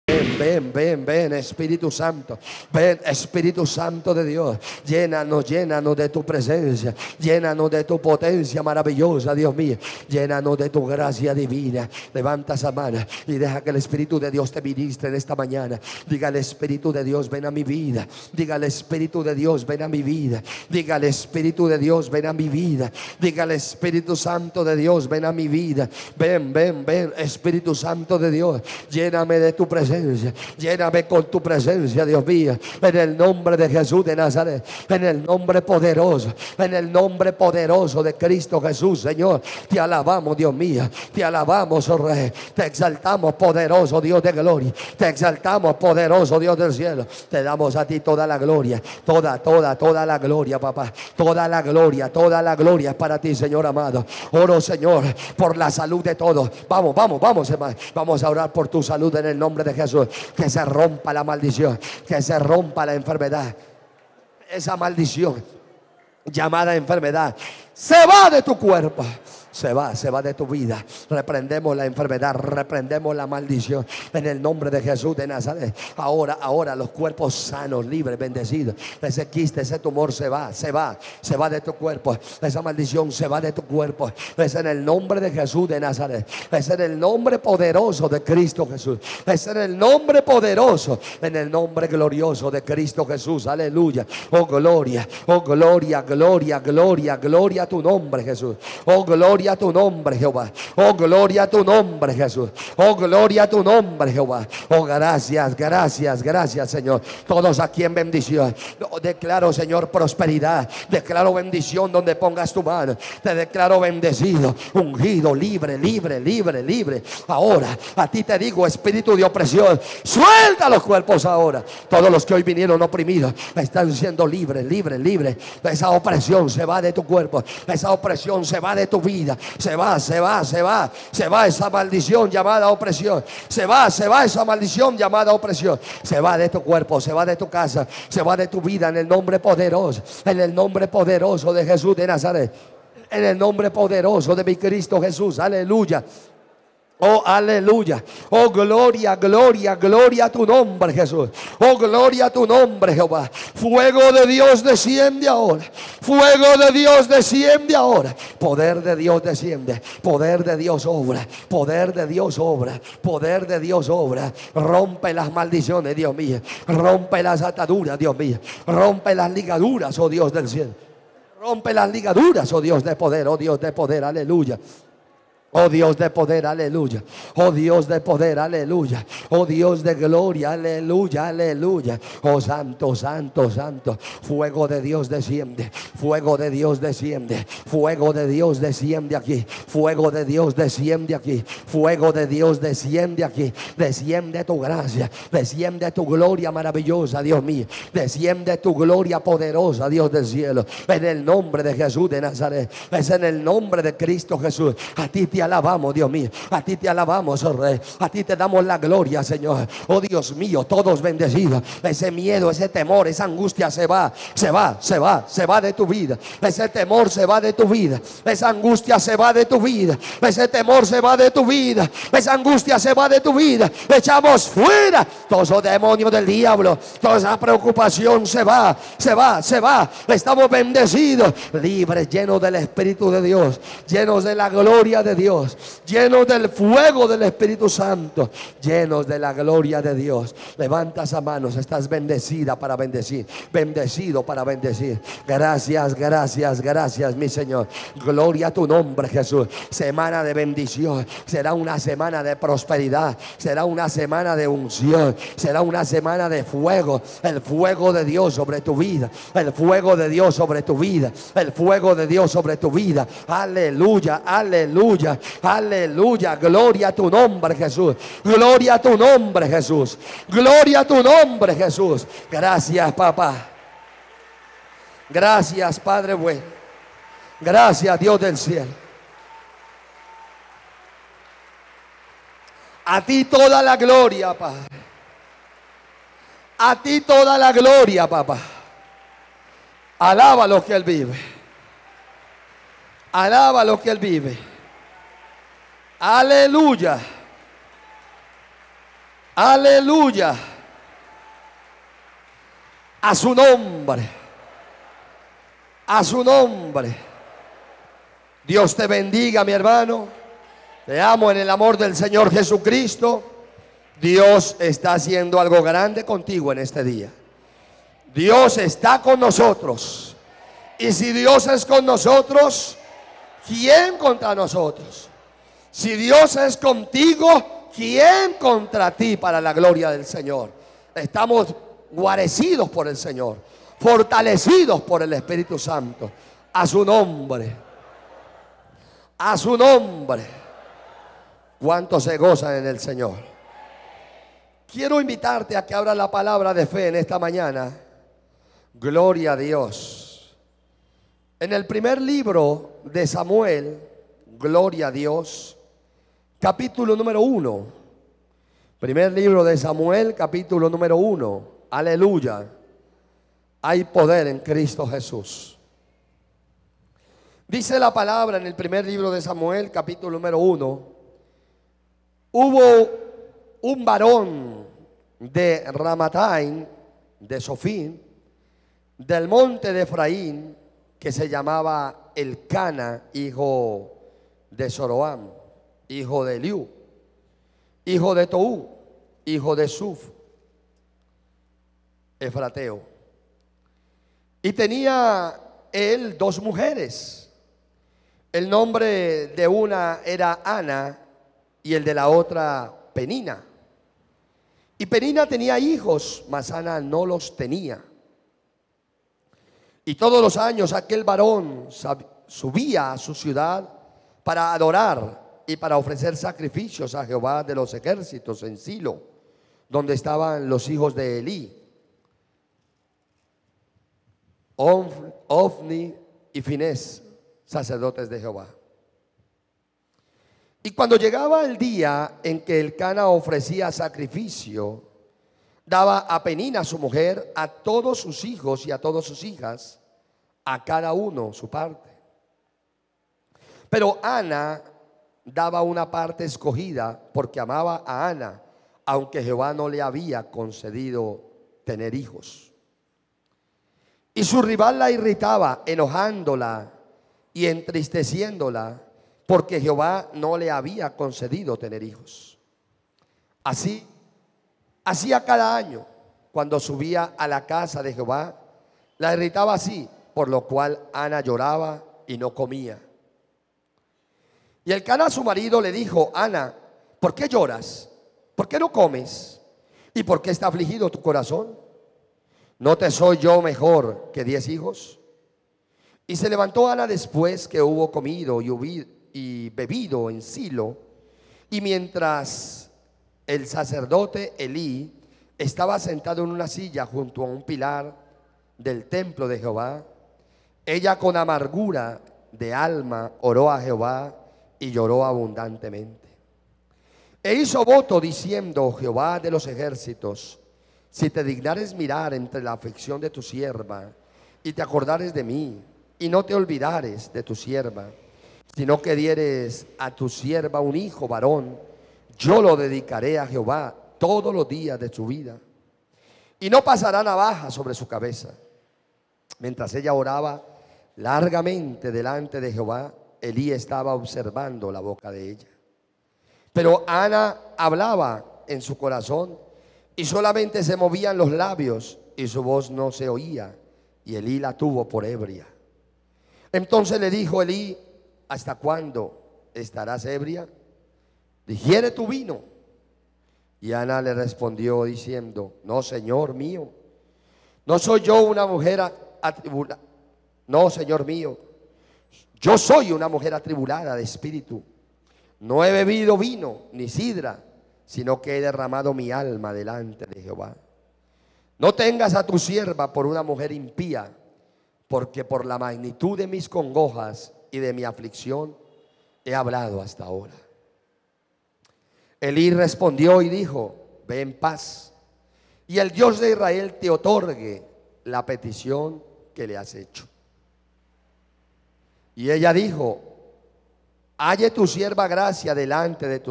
LA VERDADERA ORACIÓN 1/3 Pastor Rosemberg Olivares
DEFINITIVAMENTE NECESITAMOS APRENDER A ORAR COMO DIOS QUIERE QUE LO HAGAMOS, TEN INVITO A QUE ESCUCHES ESTA PREDICACIÓN Y TAMBIÉN A QUE LA COMPARTAS CON OTRAS PERSONAS QUE TE LO AGRADECERAN.